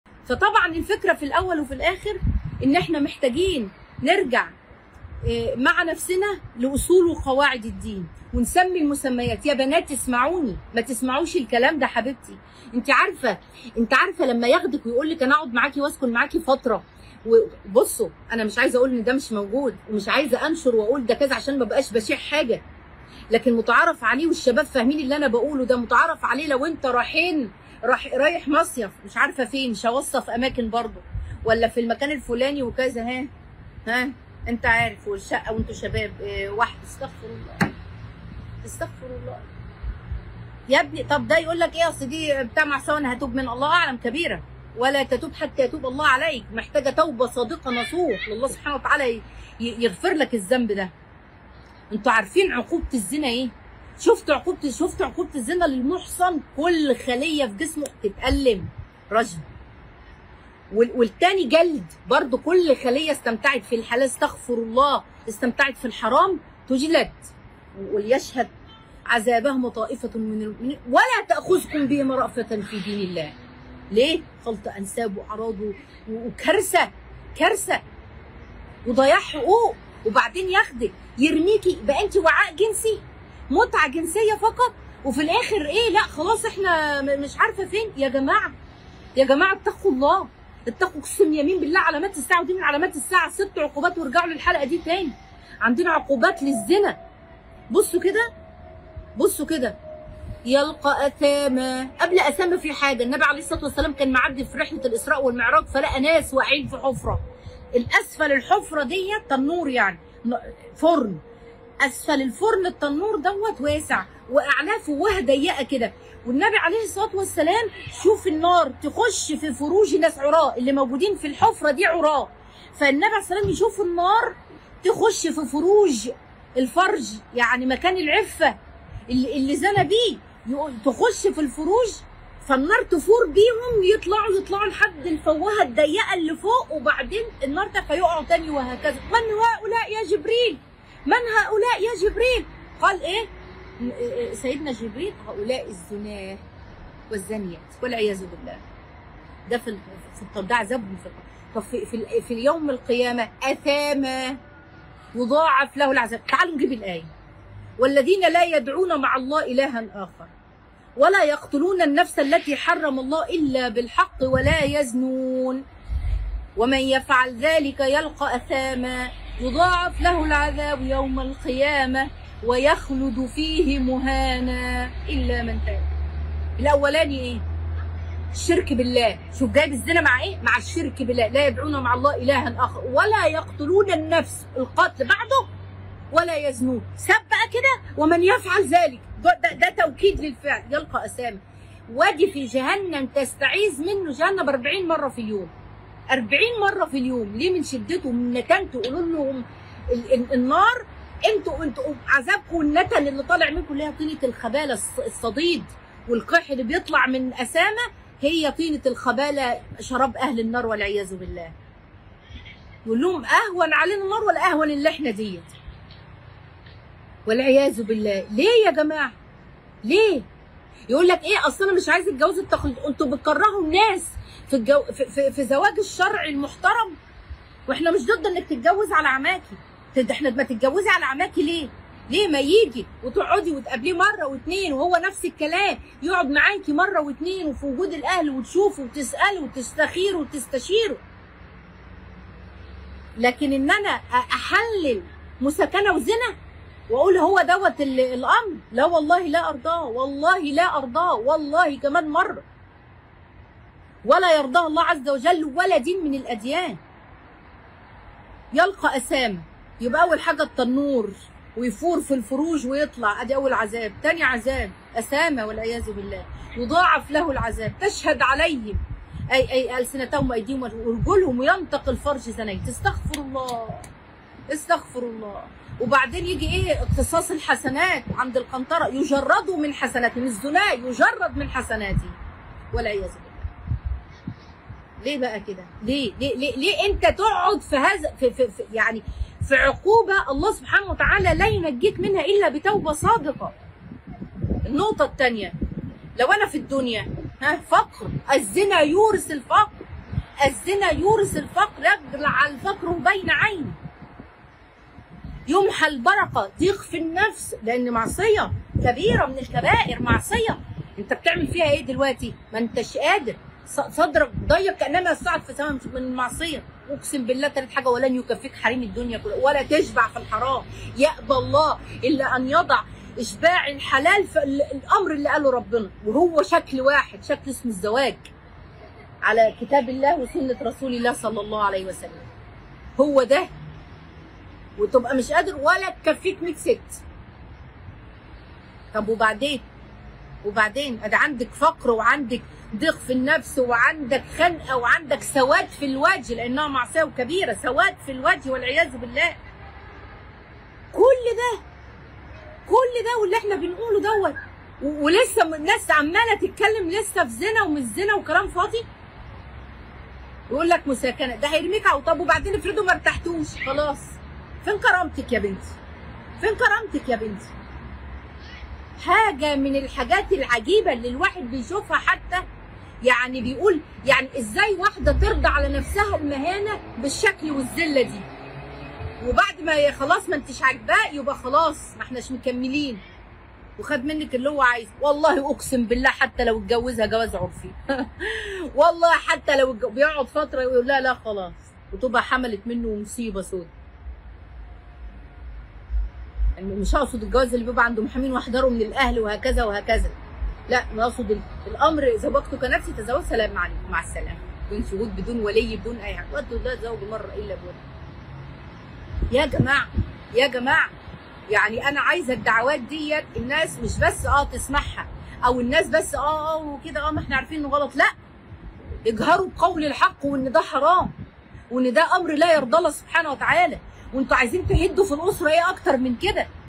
المساكنة إهانة للمرأة "المرأة مش كوب جنـ .ـسي، مترخصيش نفسك، تعرف علي عقوبة الزنـ ـا مع د. هالة سمير
shorturl.at/afwD9